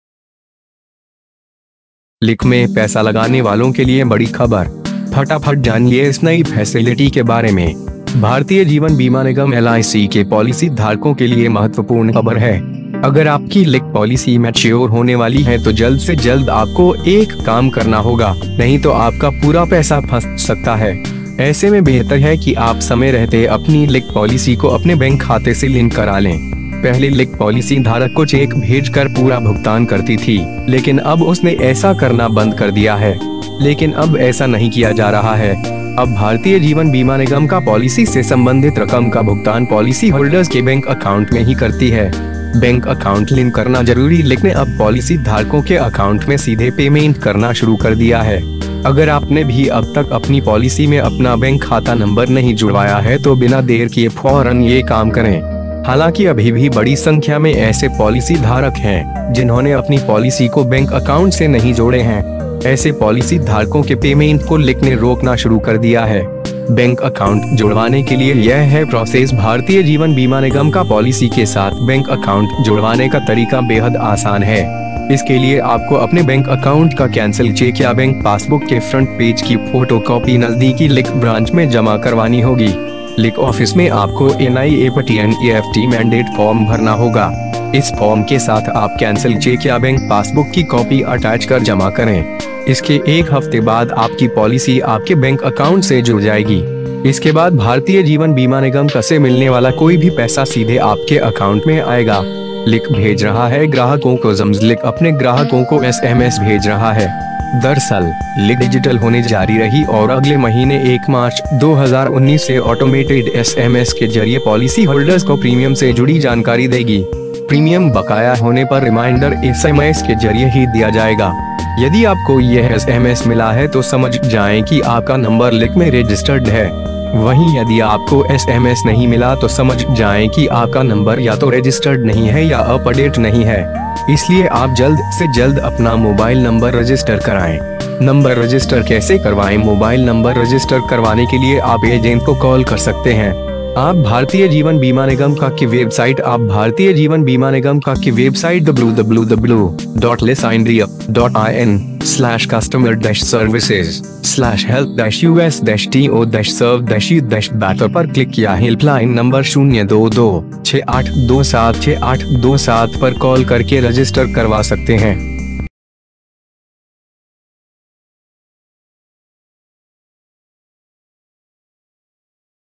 Lic में पैसा लगाने वालों के लिए बड़ी खबर! फटाफट जानिए इस नई फैसेलिटी के बारे में
भारतीय जीवन बीमा निगम (LIC) के पॉलिसी धारकों के लिए महत्वपूर्ण खबर है. अगर आपकी LIC पॉलिसी मैच्योर होने वाली है तो जल्द से जल्द आपको एक काम करना होगा, नहीं तो आपका पूरा पैसा फंस सकता है. ऐसे में बेहतर है कि आप समय रहते अपनी LIC पॉलिसी को अपने बैंक खाते से लिंक करा लें. पहले LIC पॉलिसी धारक को चेक भेजकर पूरा भुगतान करती थी, लेकिन अब उसने ऐसा करना बंद कर दिया है. लेकिन अब ऐसा नहीं किया जा रहा है. अब एलआईसी पॉलिसी से संबंधित रकम का भुगतान पॉलिसी होल्डर्स के बैंक अकाउंट में ही करती है.

बैंक अकाउंट लिंक करना जरूरी- LIC ने अब पॉलिसी धारकों के अकाउंट में सीधे पेमेंट करना शुरू कर दिया है. अगर आपने भी अब तक अपनी पॉलिसी में अपना बैंक खाता नंबर नहीं जुड़वाया है तो बिना देर किए फौरन ये काम करें. हालांकि अभी भी बड़ी संख्या में ऐसे पॉलिसी धारक हैं, जिन्होंने अपनी पॉलिसी को बैंक अकाउंट से नहीं जोड़े हैं. ऐसे पॉलिसी धारकों के पेमेंट को LIC ने रोकना शुरू कर दिया है.

बैंक अकाउंट जुड़वाने के लिए यह है प्रॉसेस- एलआईसी पॉलिसी के साथ बैंक अकाउंट जुड़वाने का तरीका बेहद आसान है. इसके लिए आपको अपने बैंक अकाउंट का कैंसिल चेक या बैंक पासबुक के फ्रंट पेज की फोटोकॉपी नजदीकी LIC ब्रांच में जमा करवानी होगी. LIC ऑफिस में आपको एनईएफटी (NEFT) मैंडेट फॉर्म भरना होगा. इस फॉर्म के साथ आप कैंसिल चेक या बैंक पासबुक की कॉपी अटैच कर जमा करें. इसके 1 हफ्ते बाद आपकी पॉलिसी आपके बैंक अकाउंट से जुड़ जाएगी. इसके बाद एलआईसी से मिलने वाला कोई भी पैसा सीधे आपके अकाउंट में आएगा.

LIC भेज रहा है ग्राहकों को SMS- LIC अपने ग्राहकों को SMS भेज रहा है. दरअसल, LIC डिजिटल होने जारी रही और अगले महीने 1 मार्च 2019 से ऑटोमेटेड SMS के जरिए पॉलिसी होल्डर्स को प्रीमियम से जुड़ी जानकारी देगी. प्रीमियम बकाया होने पर रिमाइंडर एसएमएस के जरिए ही दिया जाएगा. यदि आपको यह SMS मिला है तो समझ जाएं कि आपका नंबर LIC में रजिस्टर्ड है. वहीं यदि आपको SMS नहीं मिला तो समझ जाएं कि आपका नंबर या तो रजिस्टर्ड नहीं है या अपडेट नहीं है. इसलिए आप जल्द से जल्द अपना मोबाइल नंबर रजिस्टर कराएं.

नंबर रजिस्टर कैसे करवाएं- मोबाइल नंबर रजिस्टर करवाने के लिए आप एजेंट को कॉल कर सकते हैं. आप एलआईसी की वेबसाइट आप एलआईसी की वेबसाइट पर क्लिक या हेल्पलाइन नंबर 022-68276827 पर कॉल करके रजिस्टर करवा सकते हैं.